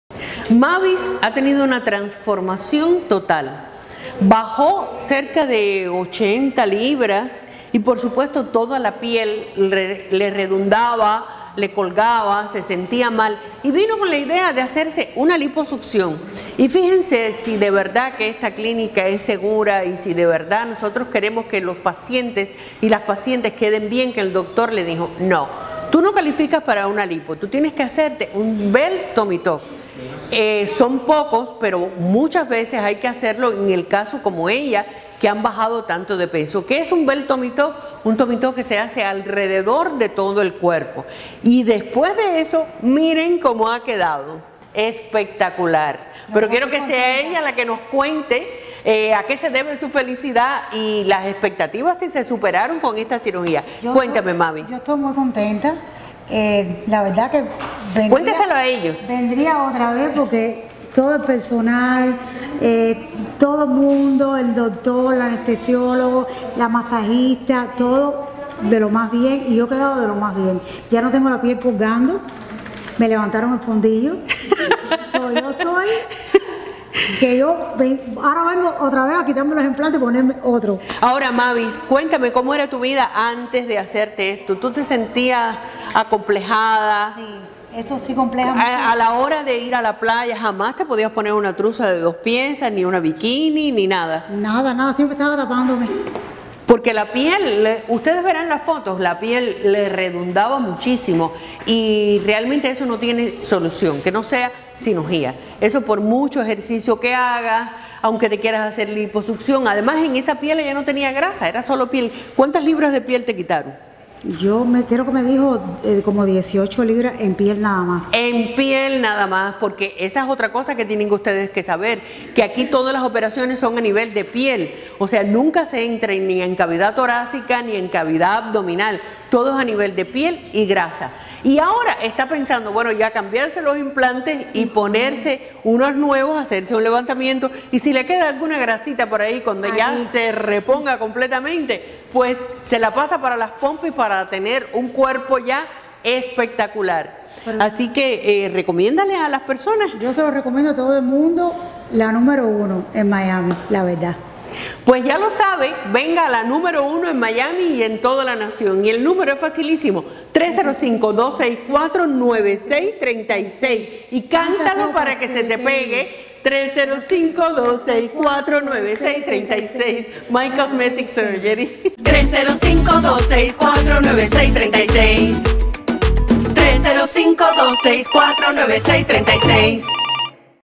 Mavis se realizó un Tummy Tuck extendido y está feliz en My Cosmetic Surgery.
La "maquinita" de My Cosmetic Surgery le hizo realidad sus sueños.